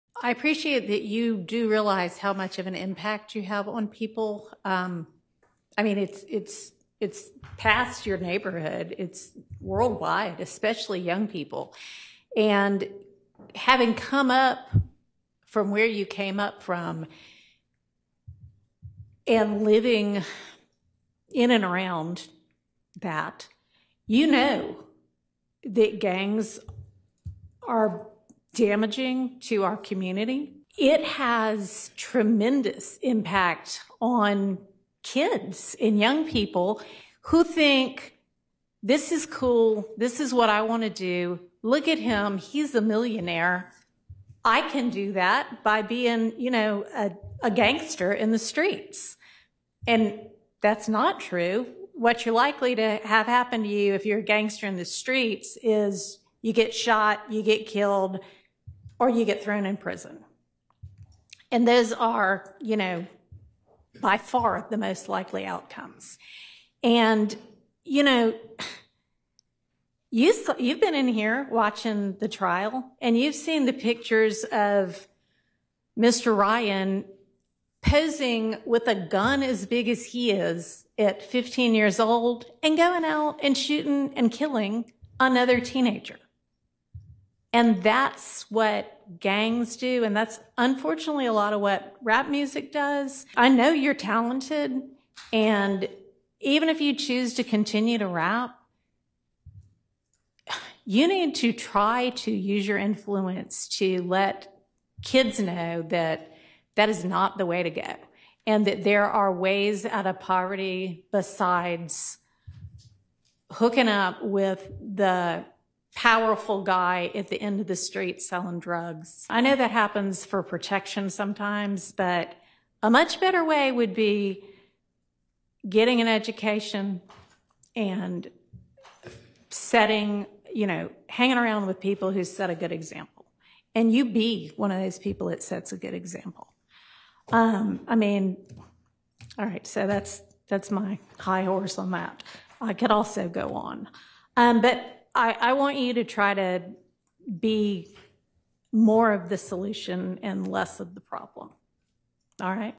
Young Thug Libertado! Após 2 Anos na Prisão, Agora em Liberdade Condicional – Impacto e Reação
Young Thug foi libertado após cumprir dois anos e meio de prisão e agora enfrentará 15 anos em liberdade condicional! Após uma longa batalha judicial e momentos tensos no tribunal, o rapper americano, conhecido globalmente pela sua influência no hip hop e cultura pop, finalmente está de volta. Neste vídeo, revelamos todos os detalhes sobre sua libertação, o impacto para a sua carreira e o que esperar daqui pra frente. Com uma liberdade condicional de longa duração, a vida de Young Thug promete mudanças dramáticas.

Assista até o final para saber como essa reviravolta está impactando a comunidade hip hop e o futuro do rapper!